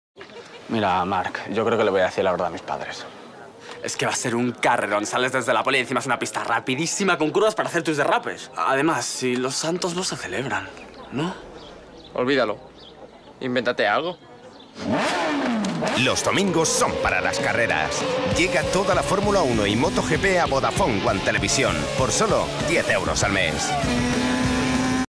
'Excusas_Márquez', de Sra Rushmore para Vodafone (Toda la Formula 1 y MotoGP)
Ficha técnica ‘Excusas (Márquez)

Anunciante: Vodafone
Marca: Vodafone
Producto:
Contacto del cliente: Cristina Barbosa, Jaime de Andrés, Susana Gimeno, Soledad Carvajal
Agencia: Sra.Rushmore.
Directores creativos ejecutivos: Miguel G. Vizcaíno, Marta Rico
Director creativo: Carlos Mañas
Redactor: Patricia Such
Director de arte: Ricardo Pastor, Ana Casado
Equipo de cuentas: Eva Gutiérrez, Jorge Moscat, Patricia de Ferrater, Mª José Martin, Henar Warncke, Alba Véliz
Planificación estratégica: Adrián Mediavilla
Equipo producción audiovisual: Santiago Romero, Isabel Gálvez
Productora: Lee Films
Realizador: Agus Berruezo
Productor ejecutivo: Angel Recio
Producer: Irma Borrell
Director de fotografía: Oriol Vila
Director de producción: Jaime Terol “Jimbo”
Estudio post-production: Serena
Título campaña: Excusas
Formatos:  25”